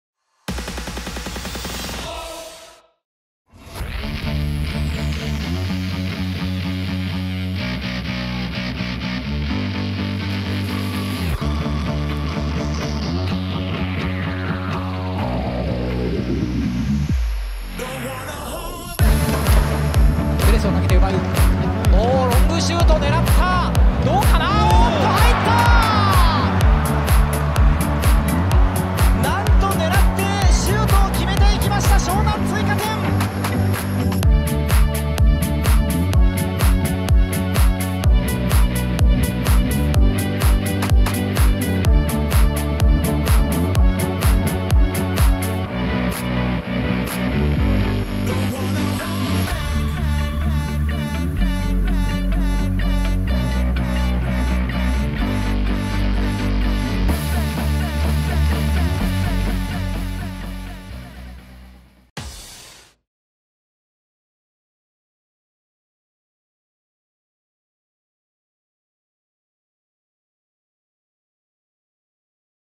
Mitsuki Saito | J1 Goal of the Season | J.LEAGUE Awards | 2020
Shonan Bellmare's 21-year-old midfielder Mitsuki Saito has been awarded the J1 Goal of the Season for 2020, for his long-range strike against Vissel Kobe - congratulations, Mitsuiki-san!